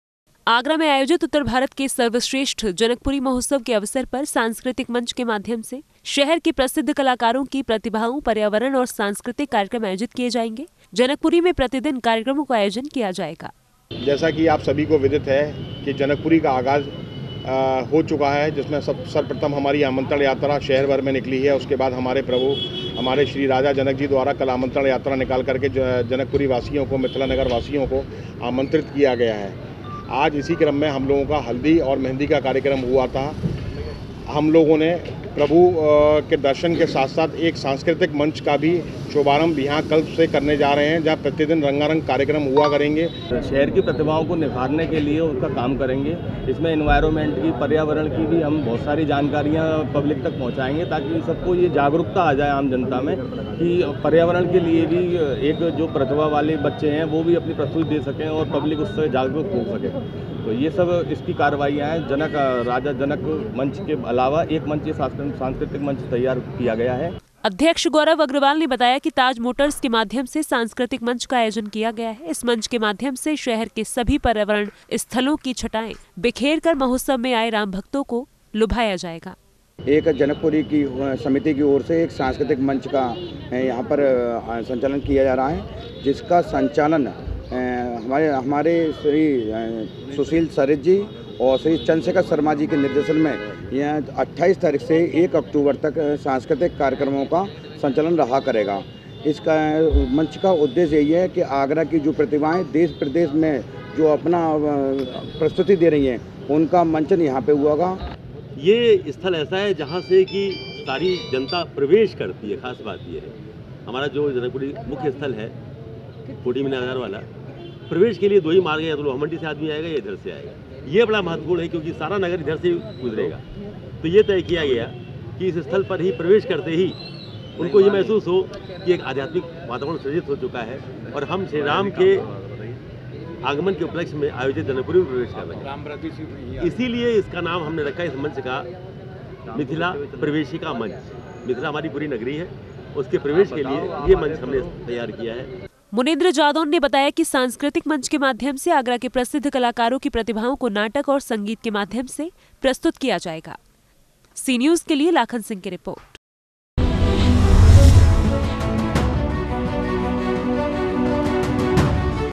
मिथिला नगरी में बनेगा सांस्कृतिक मंच | Taj Motors | famous artists | Sea News
आगरा के ताज मोटर्स के माध्यम से शुक्रवार को सांस्कृतिक मंच का आयोजन किया गया है। मंच के माध्यम से शहर के प्रसिद्ध कलाकारों के माध्यम से शहर की प्रतिभाओं,पर्यावरण,सांस्कृतिक कार्यक्रमों को प्रस्तुत किया जाएगा।